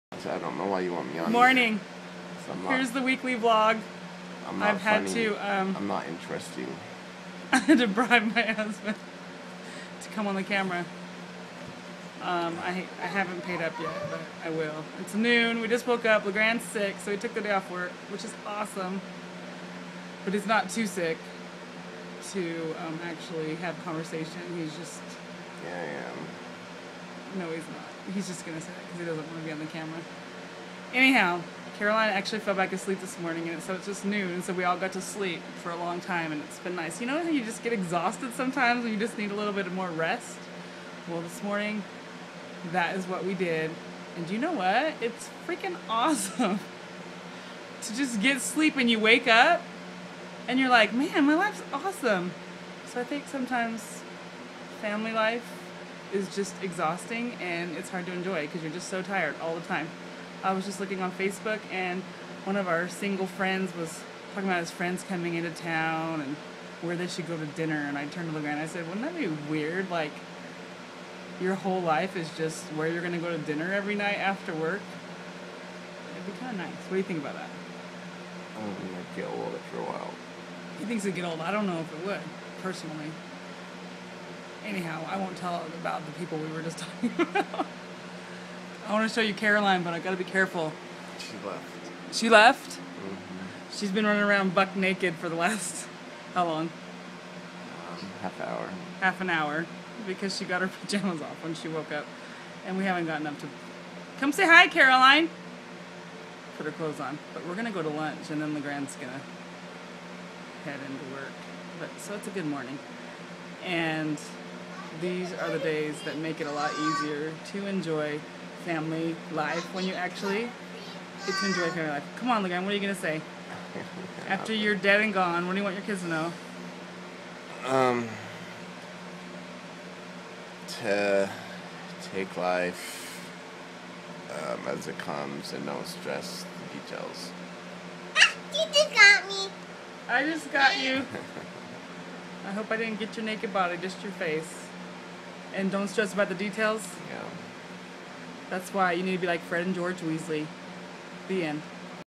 Lazy Morning
LG and I got some rest and Caroline is running around naked.